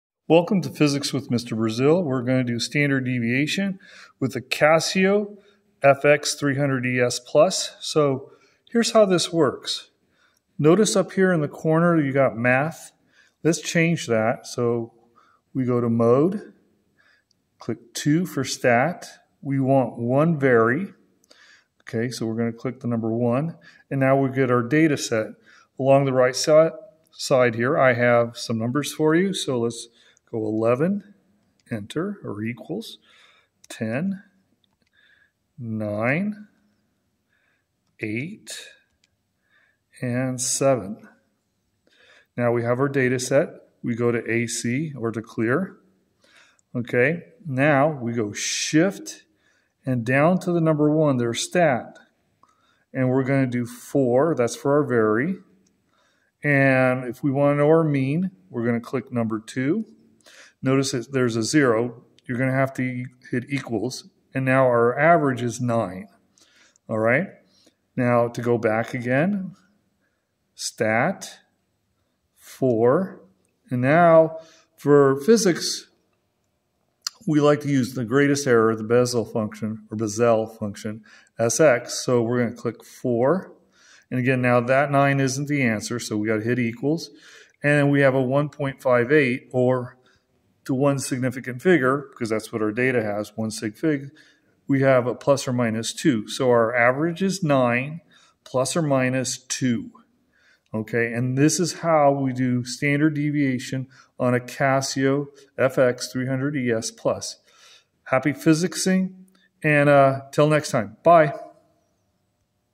Casio fx300es Plus: Standard Deviation
You will learn how to determine the mean and standard deviation using a Casio fx300es Plus calculator.